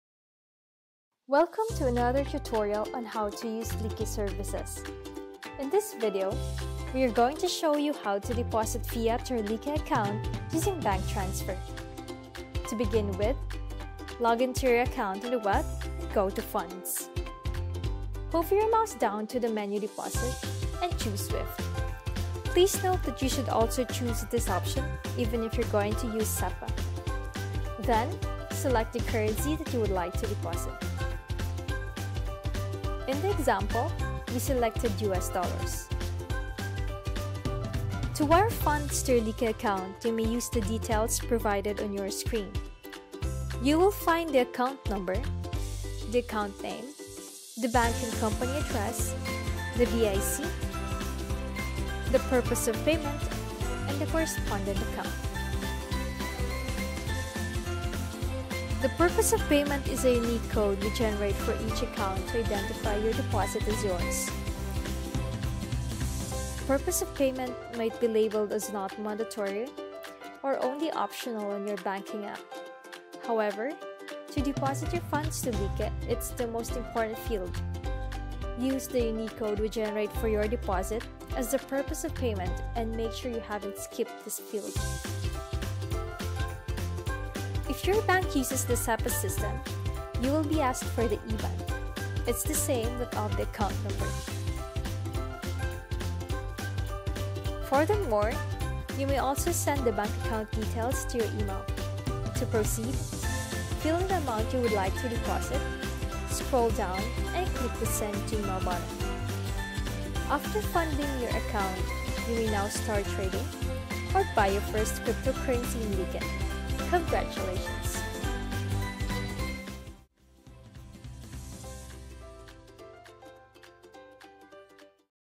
How to deposit fiat to a crypto exchange by using SWIFT or SEPA bank transfer
A how-to detailed video that explains how to use the web terminal to obtain the banking details of Lykke to deposit your fiat funds.

Lykke is an innovative digital exchange from Switzerland that offers crypto, forex, commodities and cryptoindex investing (LyCI) with a 0% trading fee.